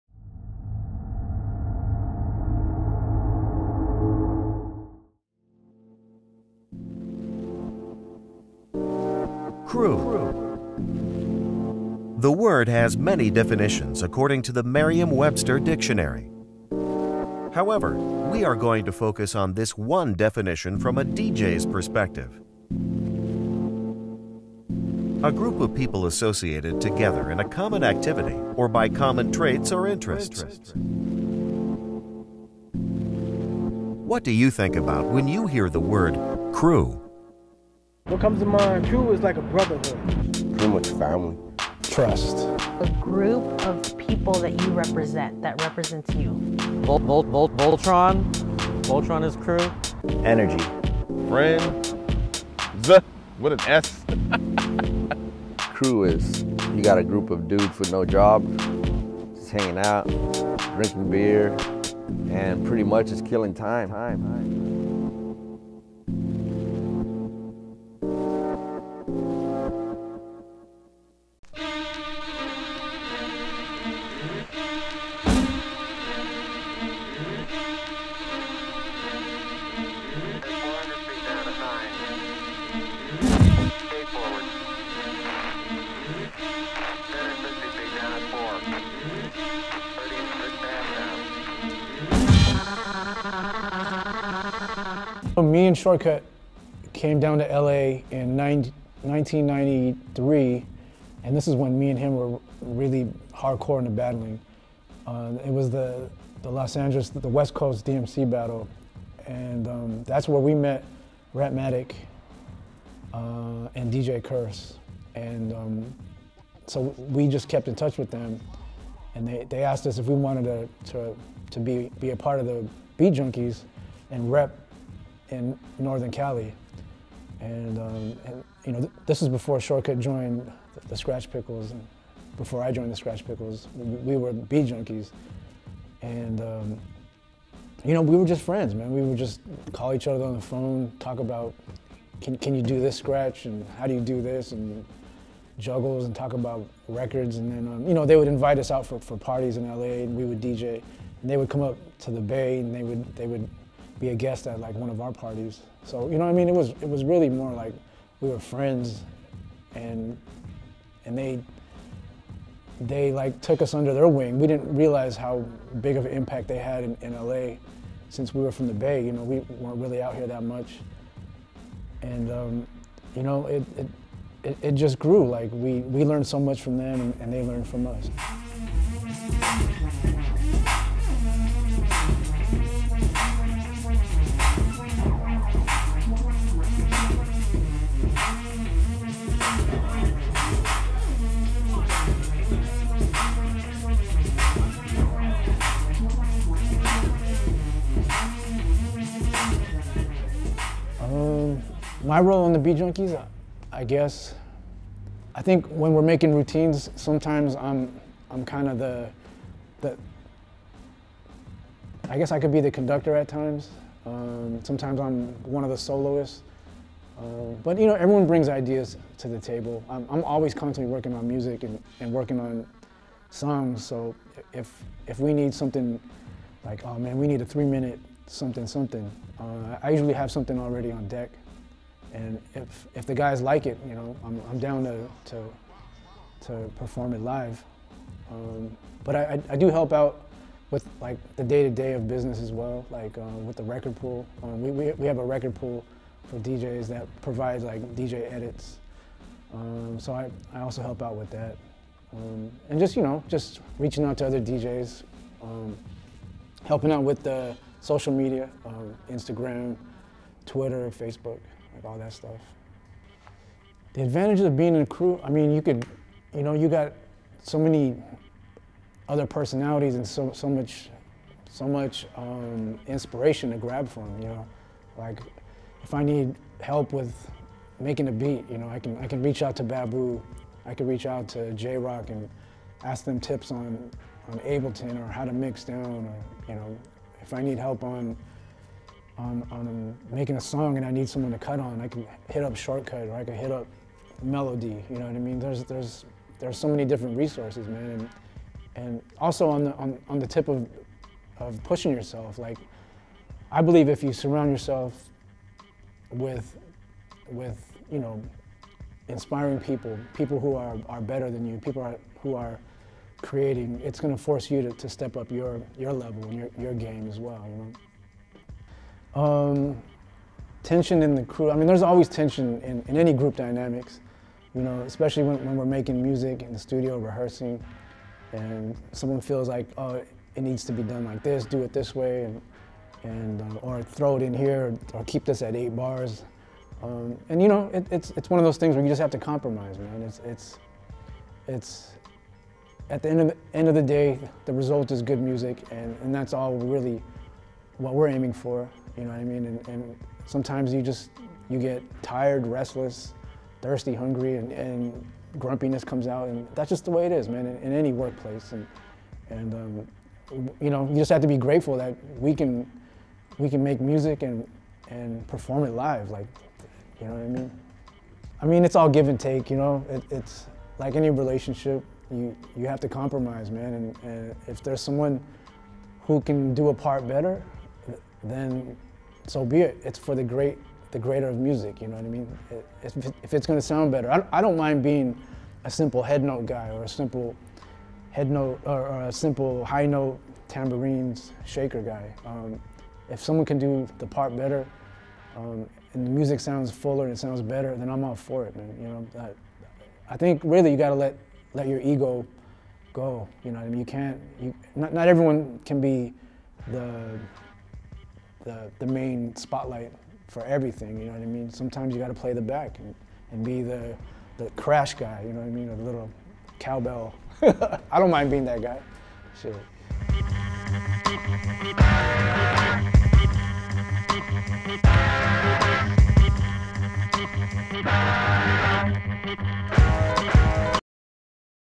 CREW Documentary - D-Styles Episode Pt. 1
The "CREW" documentary is a mini-series of DJ interviews focused on the subject of their experiences in being in a crew.  This episode is part 1 of 2 featuring D-Styles.

Credits:
Producer and Director: Alfonzo Thomas

Edited by: Alfonzo Thomas

Camera: Alfonzo Thomas and Kurt Bartolome

Photographs courtesy of Dave Paul and Juan Betts

B-Roll video footage courtesy of Matt Alhona